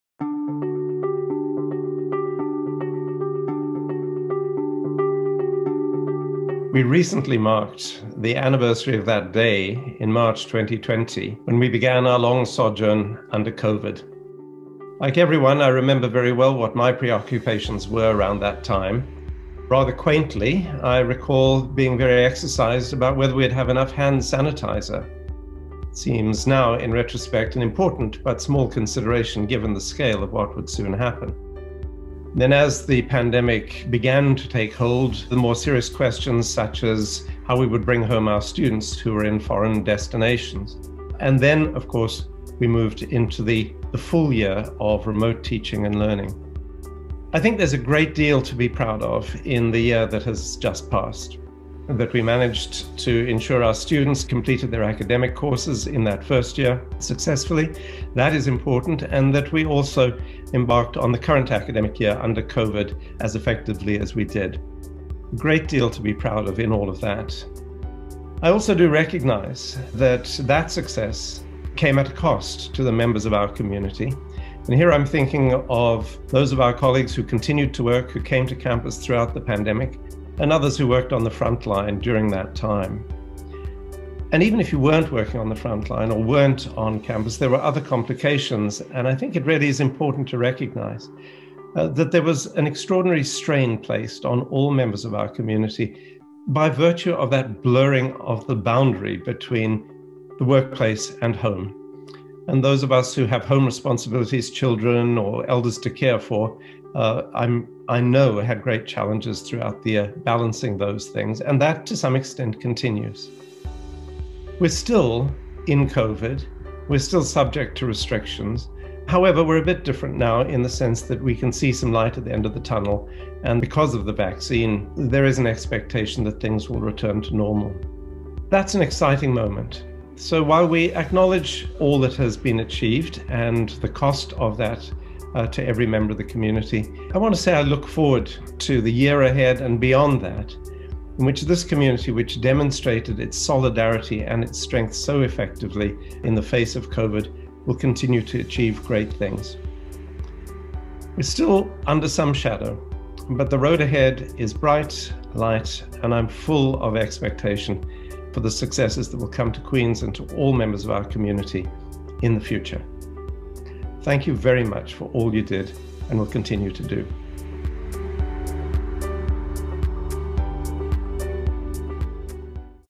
One Year Anniversary of Covid-19
Queen's University Principal Patrick Deane offers some reflection and thoughts on the first anniversary of the Covid-19 pandemic. Filmed March 10, 2021